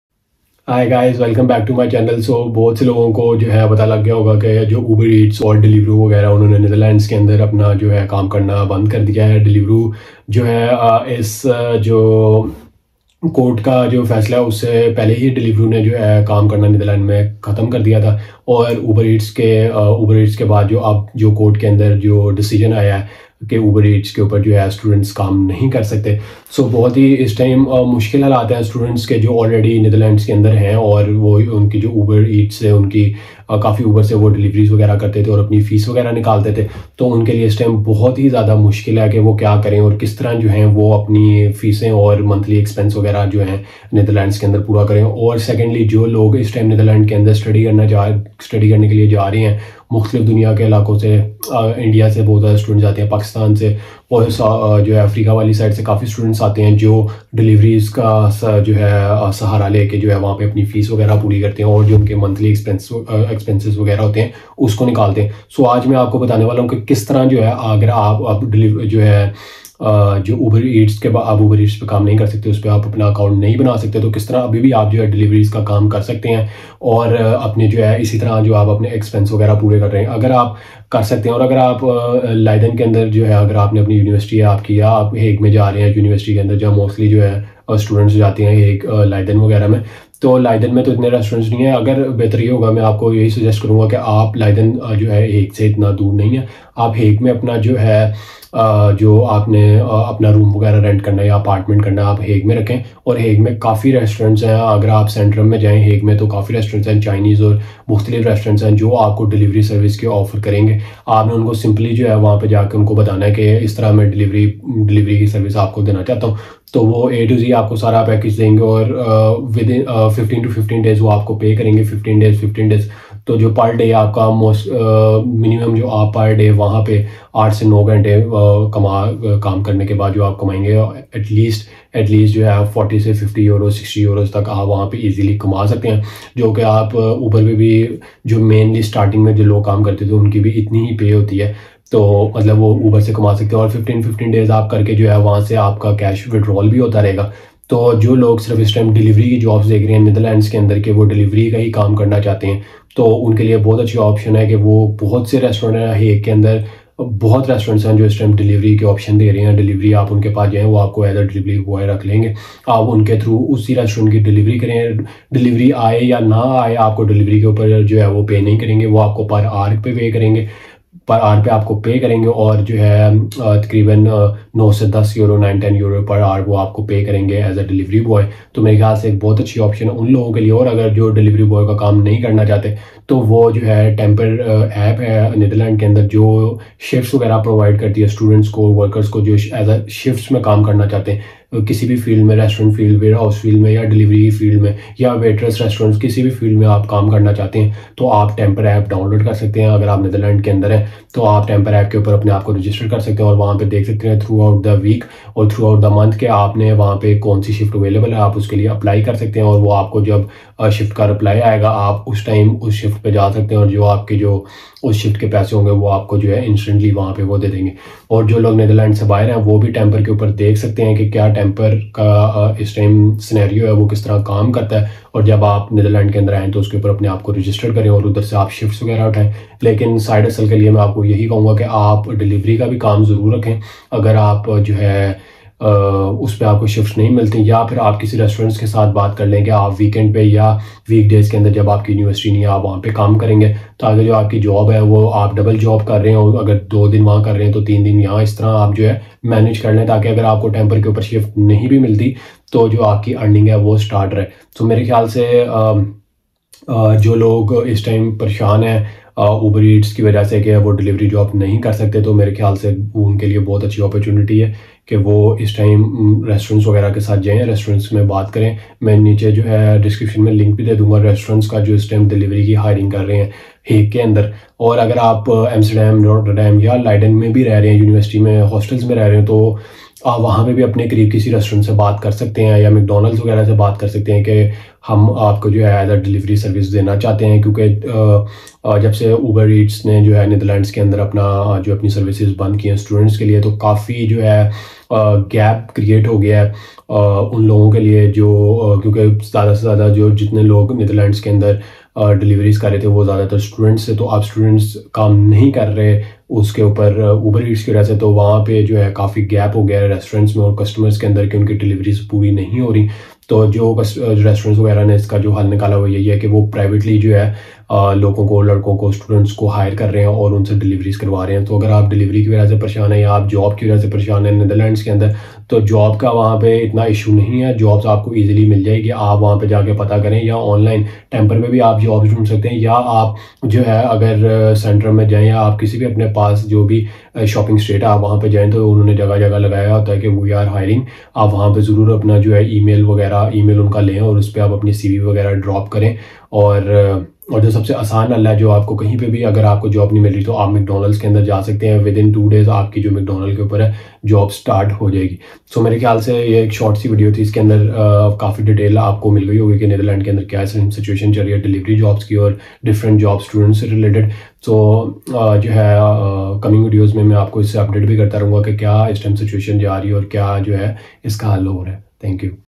Delivery Jobs In The Netherlands After Uber Eats 2023
The Netherlands is a hub for logistics and delivery, with a thriving e-commerce industry and a highly developed transportation network. As a result, there are many delivery jobs available in the country, ranging from package delivery to courier services.

Delivery jobs in the Netherlands can be found through a variety of channels, including online job boards, recruitment agencies, and direct applications to companies. Many delivery jobs require a valid driver's license and a clean driving record, as well as strong customer service skills and the ability to work independently.

Some of the most popular delivery jobs in the Netherlands include working for large delivery companies such as PostNL, DHL, and UPS, as well as local courier services. Delivery drivers may work on a full-time or part-time basis and may be responsible for delivering packages and goods to both businesses and individuals.

In addition to traditional delivery jobs, there are also opportunities to work as a bicycle courier or delivery driver for food delivery services such as after Deliveroo, Uber Eats, only thuisbezorgd is left for students. These jobs typically involve delivering food and beverages to customers on a flexible schedule, using a bicycle or scooter.

Overall, delivery jobs in the Netherlands can offer a rewarding and stable career path for those who are organized, reliable, and enjoy working independently. With the continued growth of e-commerce and online shopping, the demand for delivery drivers is only expected to increase in the coming years.

Delivery apps in the Netherlands

Websites for delivery jobs!